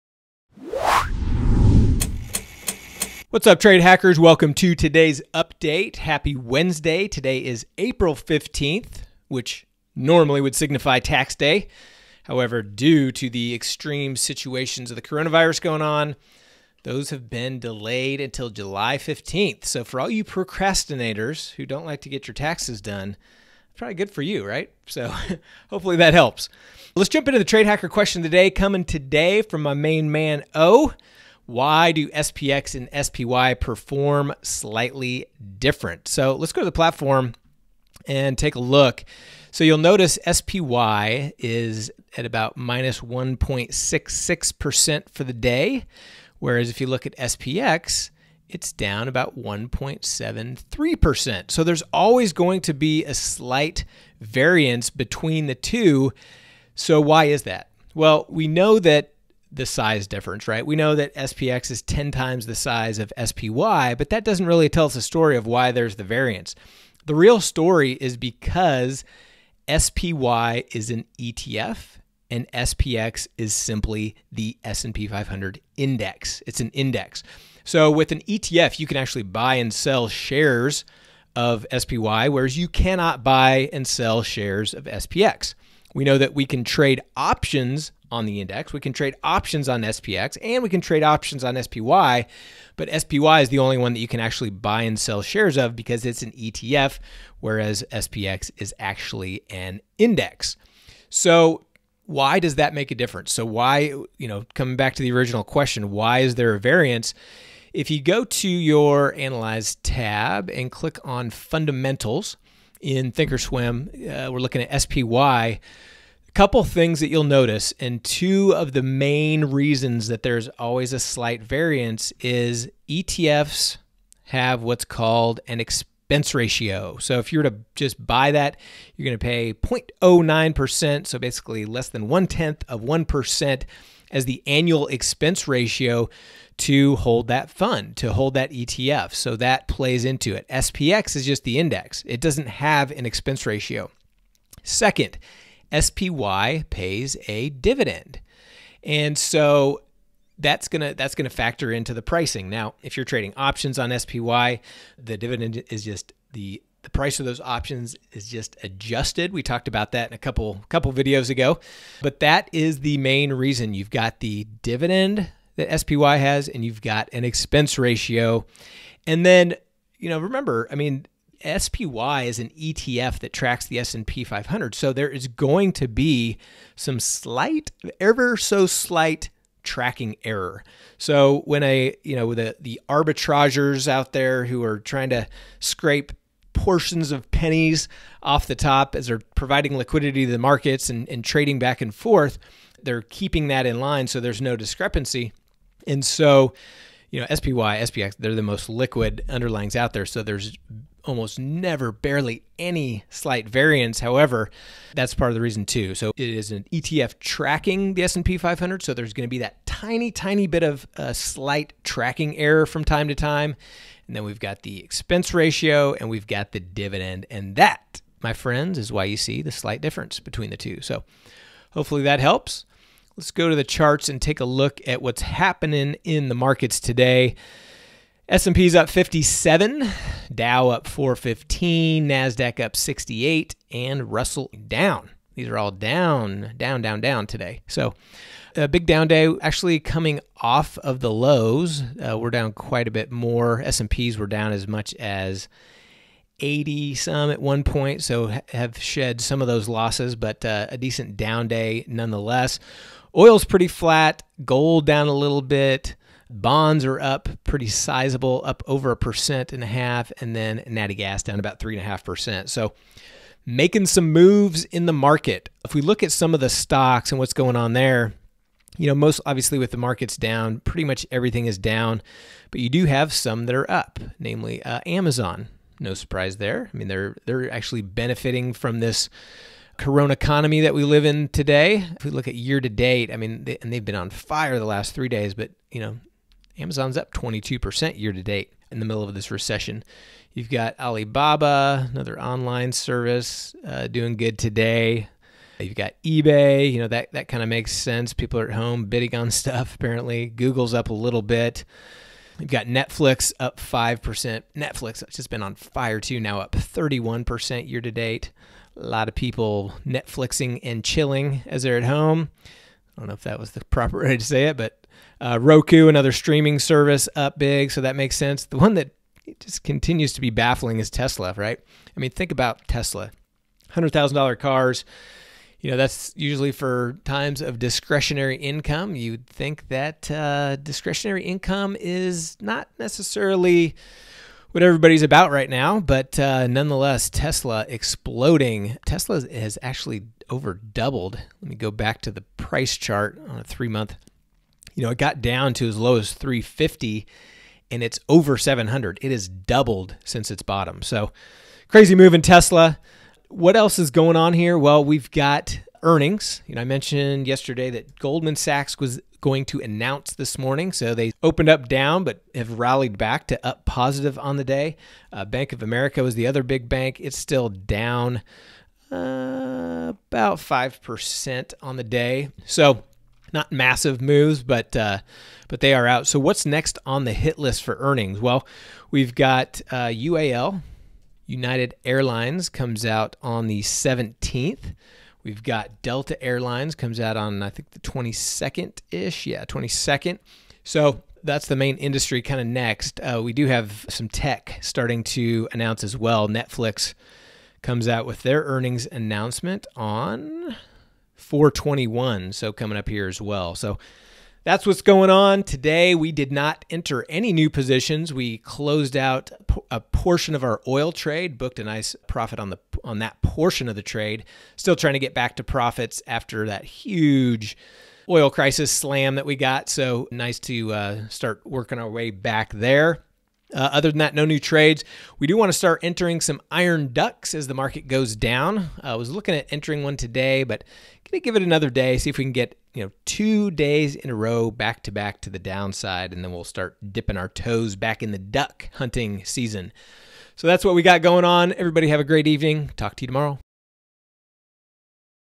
What's the Difference Between SPX & SPY? [TradeHacker Update]
Here's what we'll cover in today's update:
• What's the difference between SPX and SPY?
• Here come the airline earnings
• Bank earnings pretty mixed

Happy Trading!

YouTube.com/navigationtrading
Facebook.com/navigationtrading